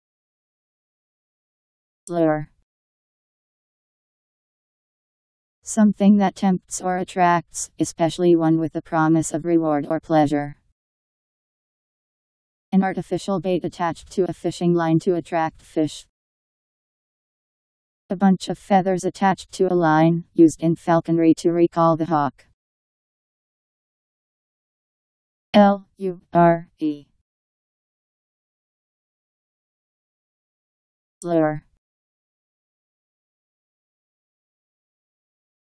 Lure Meaning
Video shows what lure means. Something that tempts or attracts, especially one with a promise of reward or pleasure.. An artificial bait attached to a fishing line to attract fish.. A bunch of feathers attached to a line, used in falconry to recall the hawk..  Lure Meaning. How to pronounce, definition audio dictionary. How to say lure. Powered by MaryTTS, Wiktionary